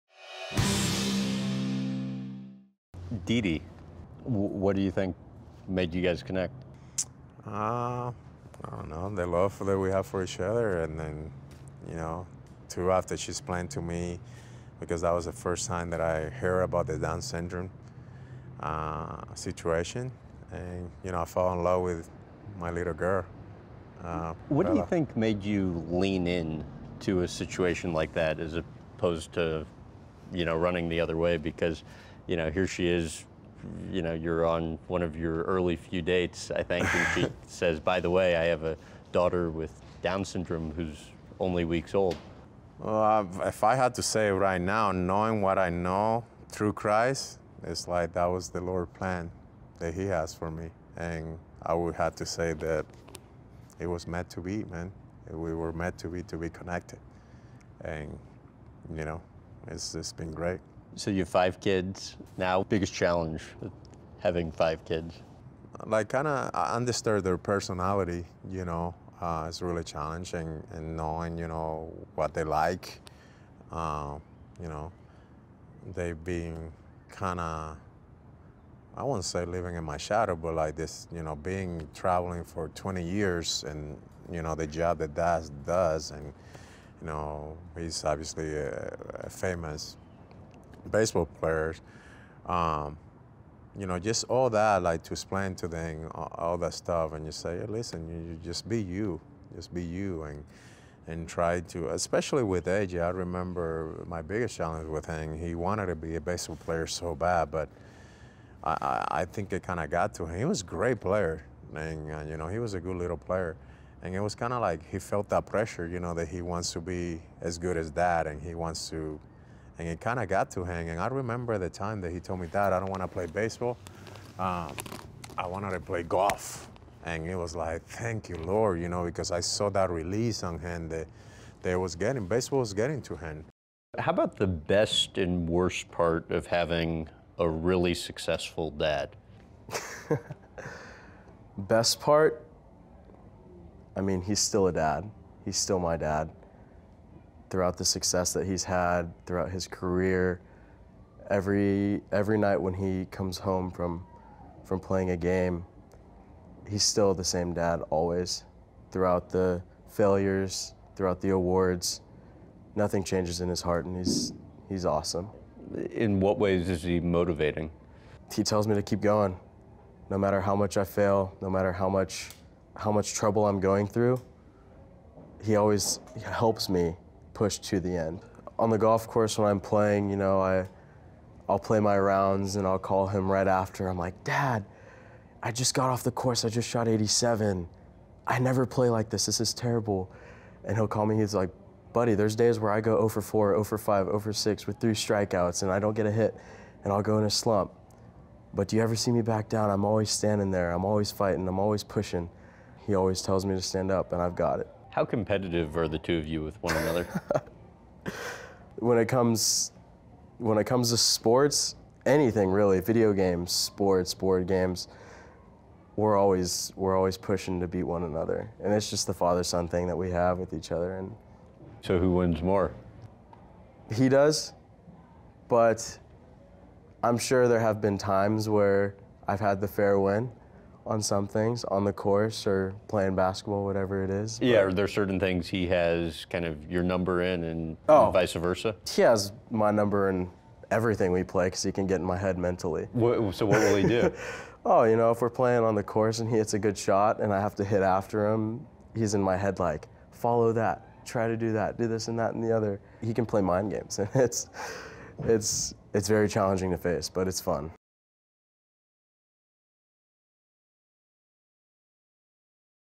Albert Pujols: Advice to my children
Albert Pujols talks about family, from his relationship with his wife, to having a daughter with Down syndrome, to the challenges of raising five kids as a celebrity athlete. Plus, his oldest son, A.J., describes his father as a constant source of motivation… and friendly competition.